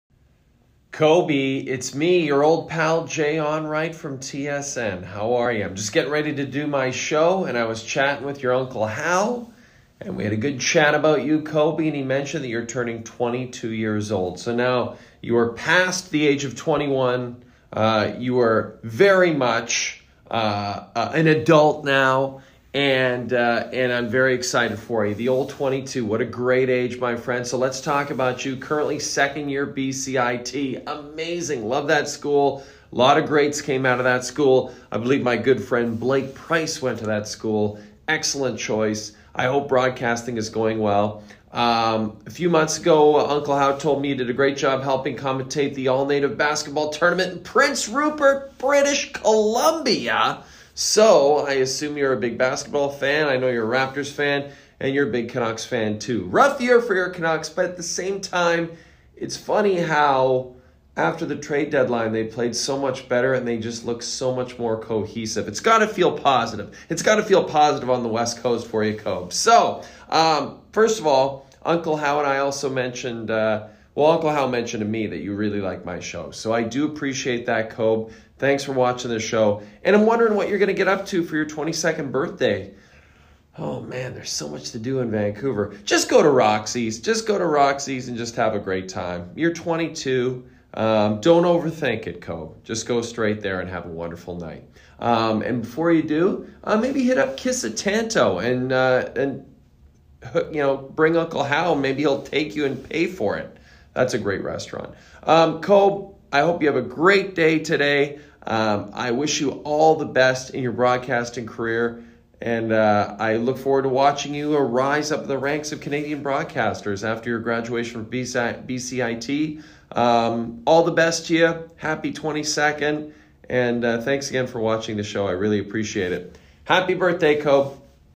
Onrait
from memmo, a birthday greeting for my nephew Kobie for his birthday